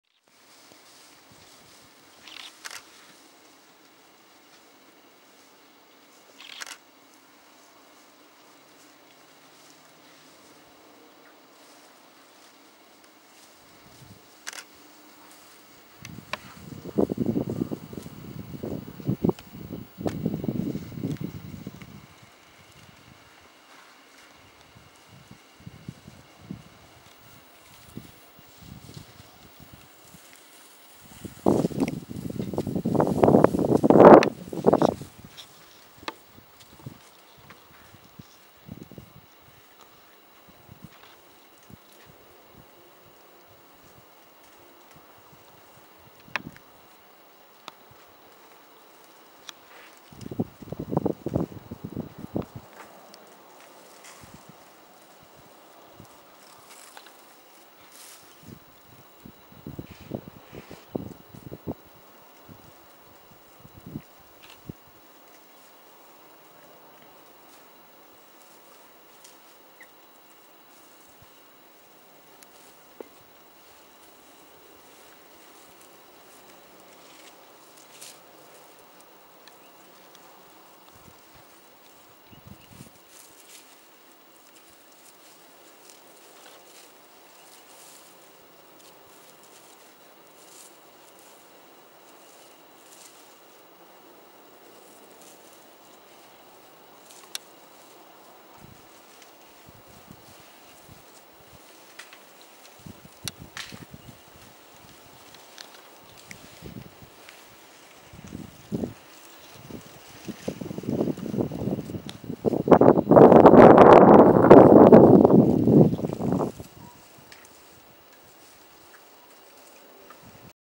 Elephant breeding herd
Probably my favourite moment from my time in South Africa. While conducting research into African wild dog behaviour last year with Wildlife ACT, I was lucky enough at ZRR to spend time in the presence of these incredible Elephants including some beautiful young calfs